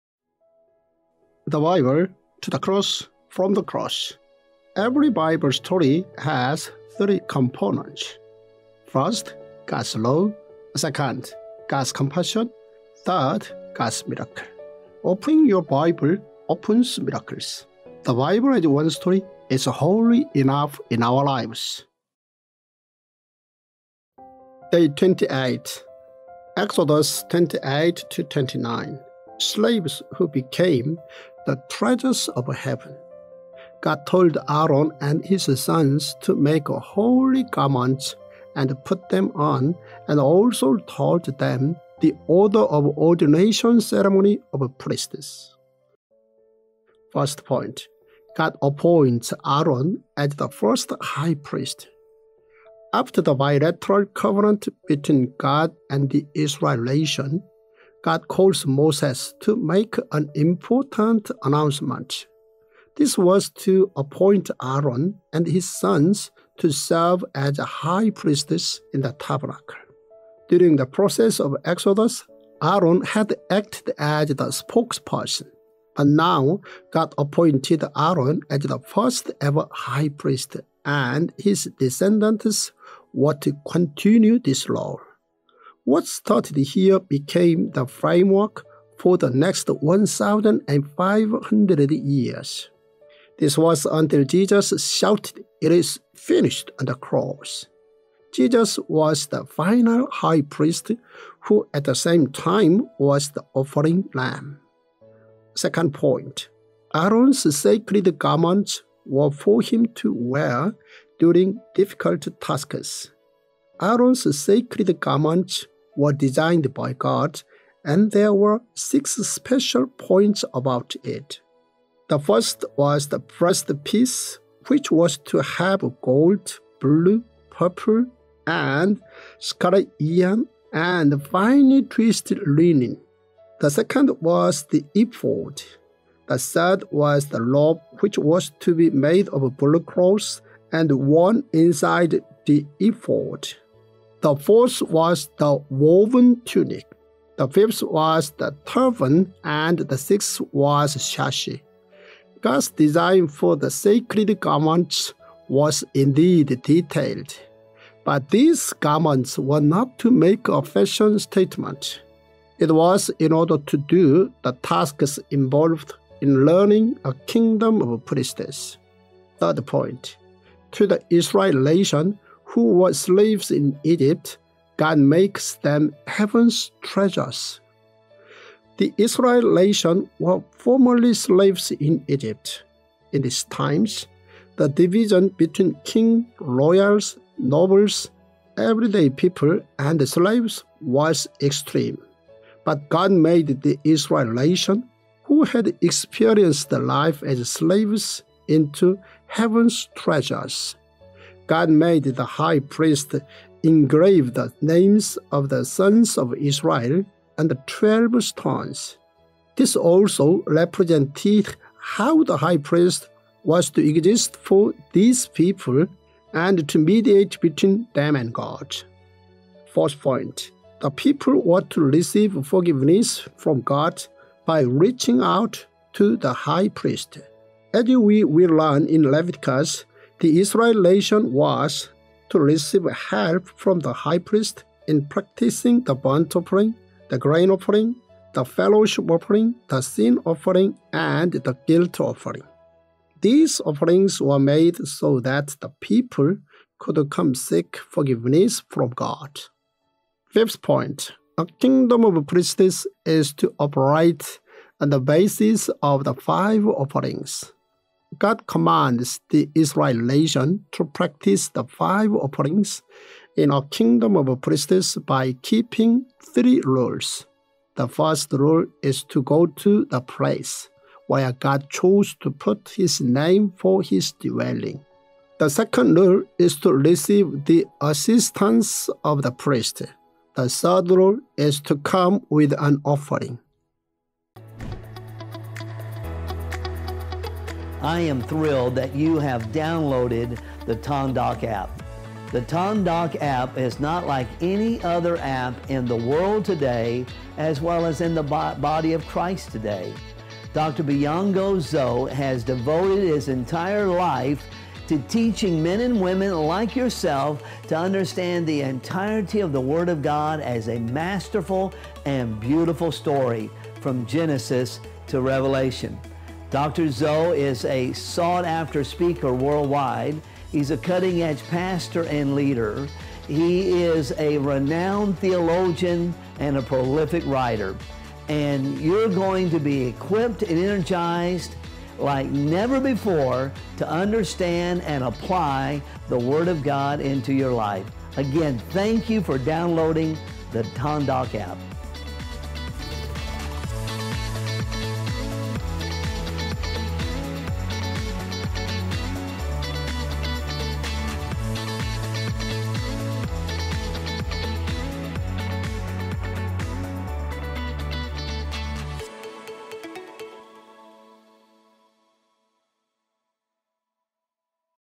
Tongdok Bible | Day 28 Exodus 28-29 | Slaves who became the treasures of heaven | Dr.Byoungho Zoh
Tongdok Bible | Day 28 (Exodus 28-29)

Slaves who became the treasures of heaven
God told Aaron and his sons to make holy garments and put them on and also told them the order of ordination ceremony of priests.